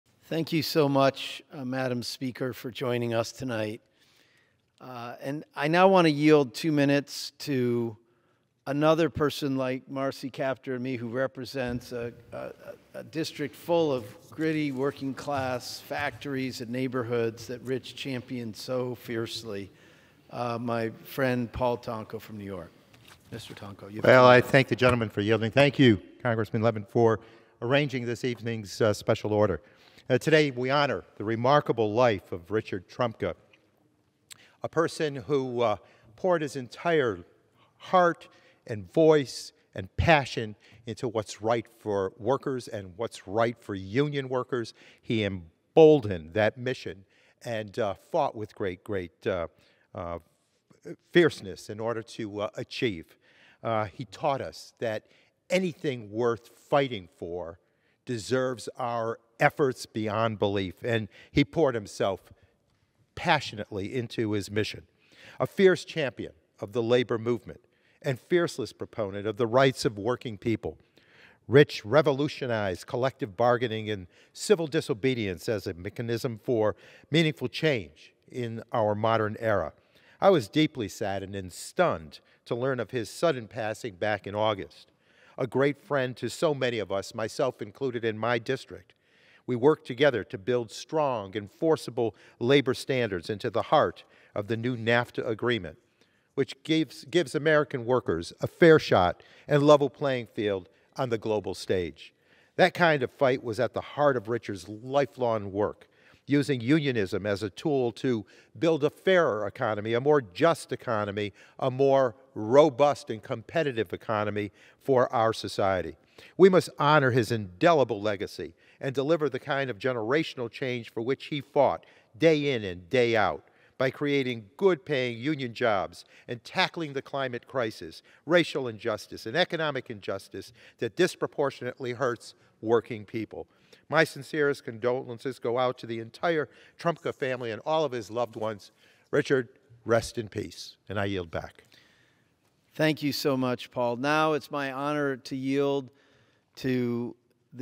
Democratic Lawmaker Speaks About Building Back Better Through Union Jobs
Rep. Paul Tonko (D-NY) speaks on the House floor about labor, Rich Trumka, and unions on Tuesday.